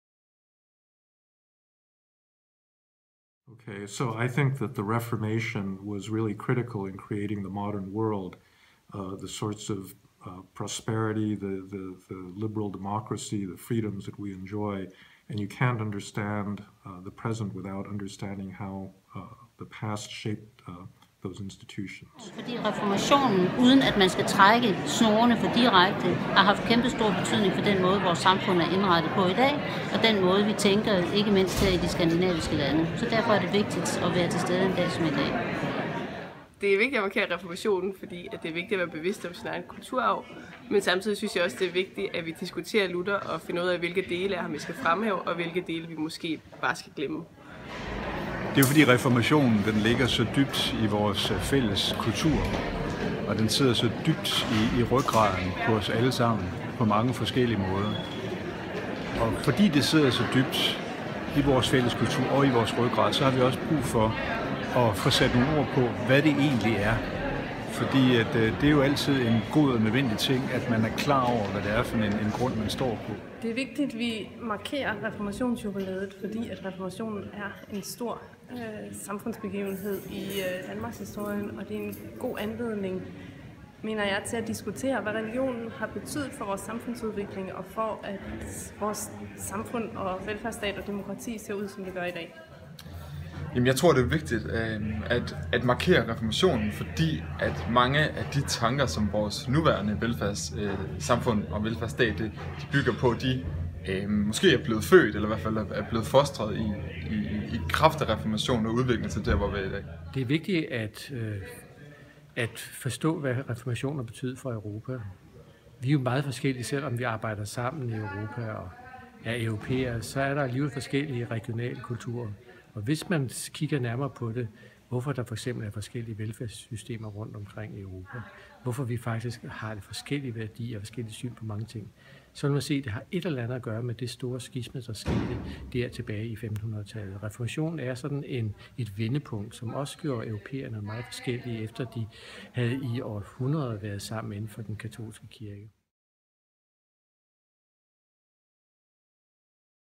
Matchpoints 2017: VOXPOP: Hvorfor er det vigtigt at markere reformationsjubilæet?
Hør Francis Fukuyama, Mette Bock, Michael Böss og en række deltagere ved Matchpoints 2017 reflektere over, hvorfor det er vigtigt at markere reformationsjubilæet.